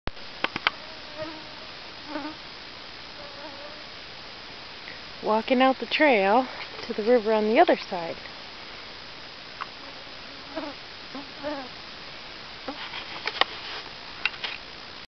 kantishna mosquitoes Alaska
I have never seen so many mosquitoes before at one time!  They were vicious!!  June 2011 at my parents place on the Kantishna